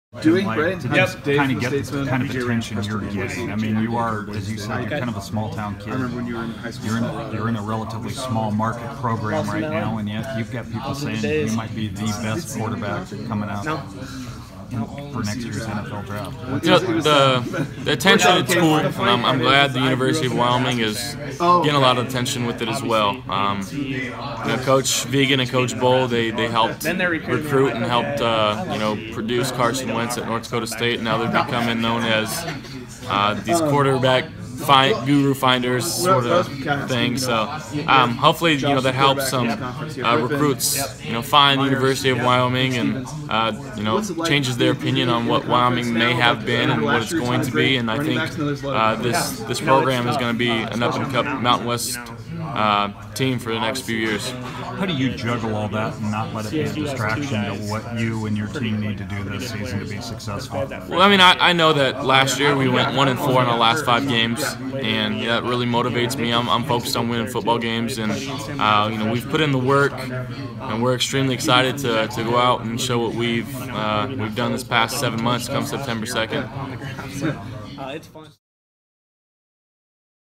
Wyoming QB Brett Allen can't hide from hype
Wyoming quarterback Josh Allen learning to deal with life in spotlight as top NFL draft prospect.